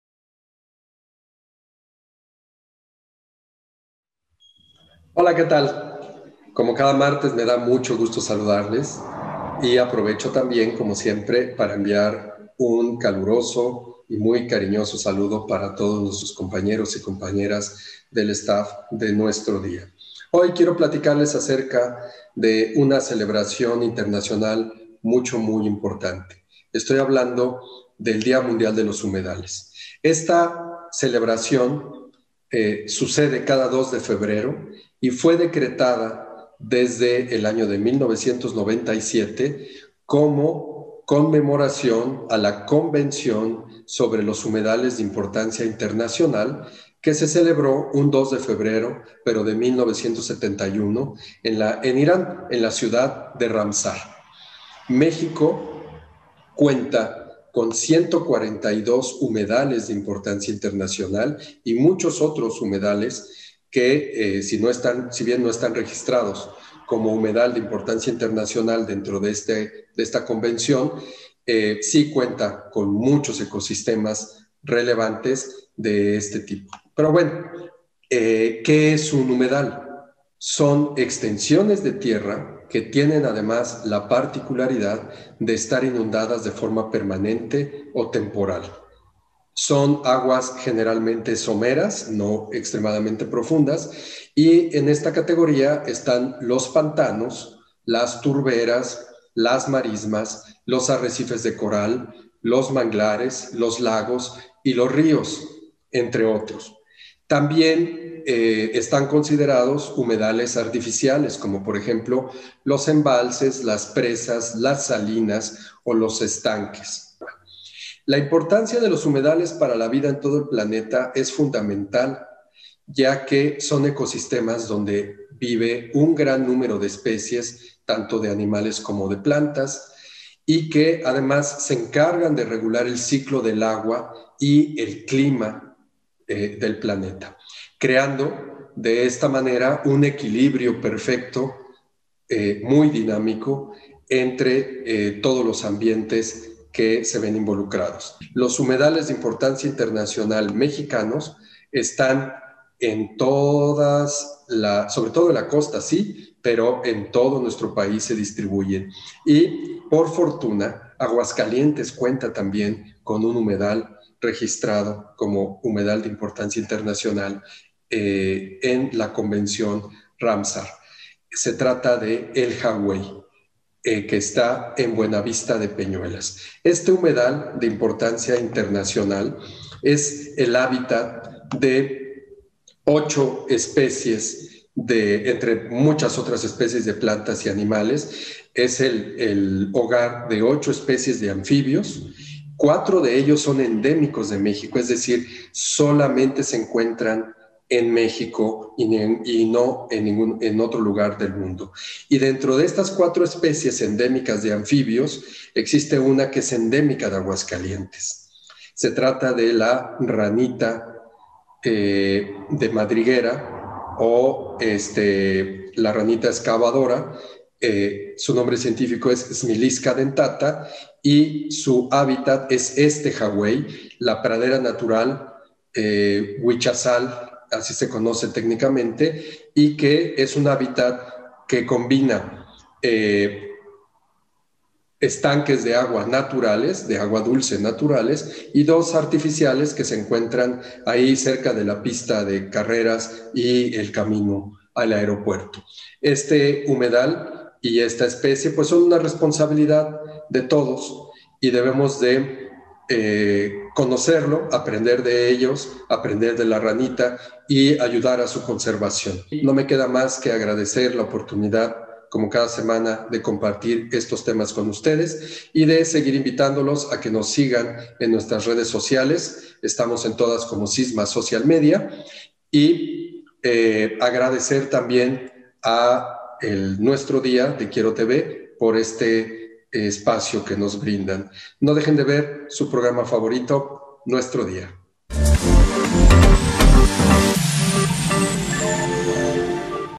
2 FEBRERO, DÍA MUNDIAL DE LOS HUMEDALES
Desde el año 1997 se conmemora este día tan especial, el cuál busca hacer conciencia sobre la vital importancia de estos ecosistemas, el Biólogo Luis Manuel Ortíz nos platica más sobre el tema.